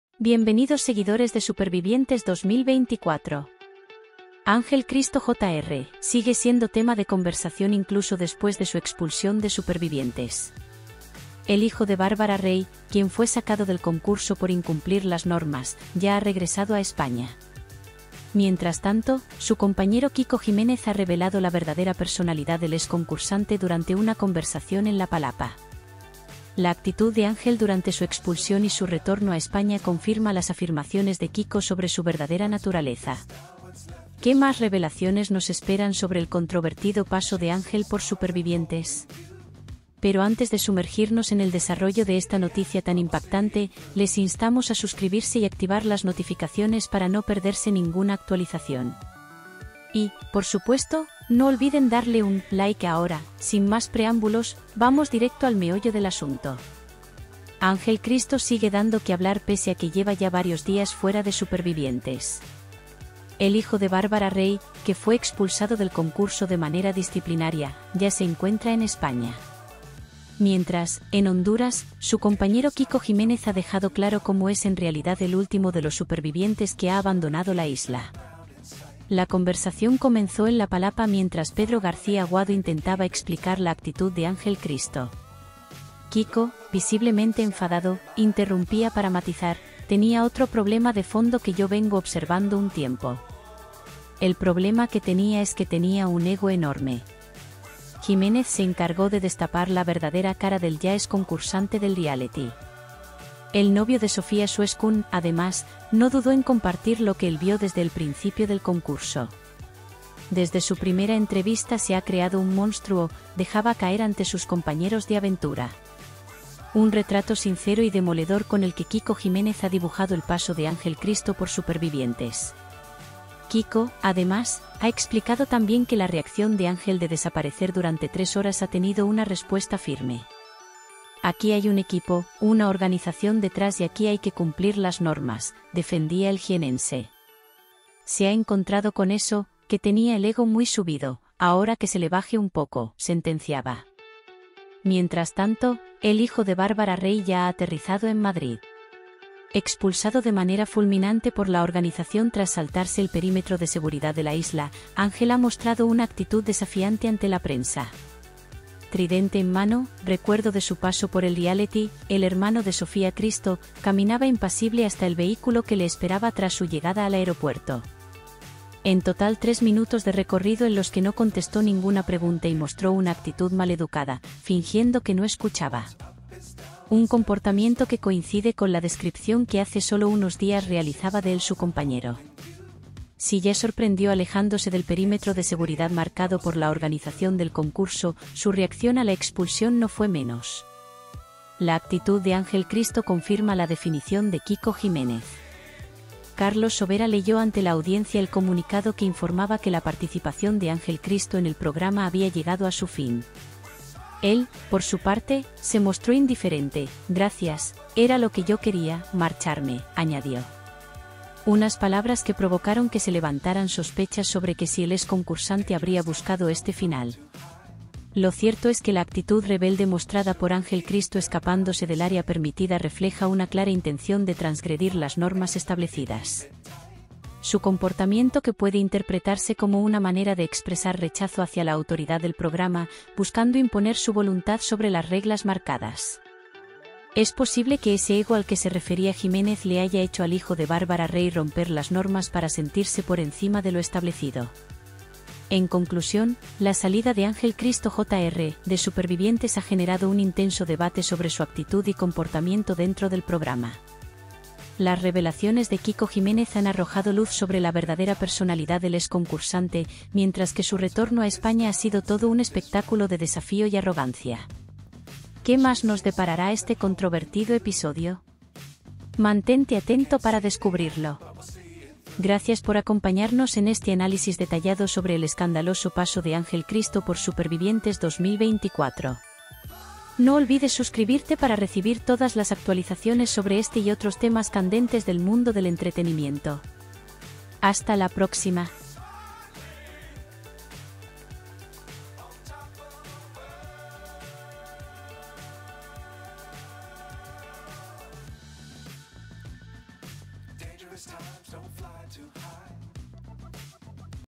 Kiko Jiménez destapa un dato inédito sobre la actitud de Ángel Cristo 'Un monstruo'
Descubre todos los detalles detrás de la polémica salida de Ángel Cristo Jr. de Supervivientes 2024. En este vídeo, analizamos las revelaciones explosivas de Kiko Jiménez sobre la verdadera actitud del exconcursante y su impactante regreso a España. ¿Qué hay detrás de este comportamiento desafiante? ¡Entérate de todo aquí y no te pierdas ni un detalle!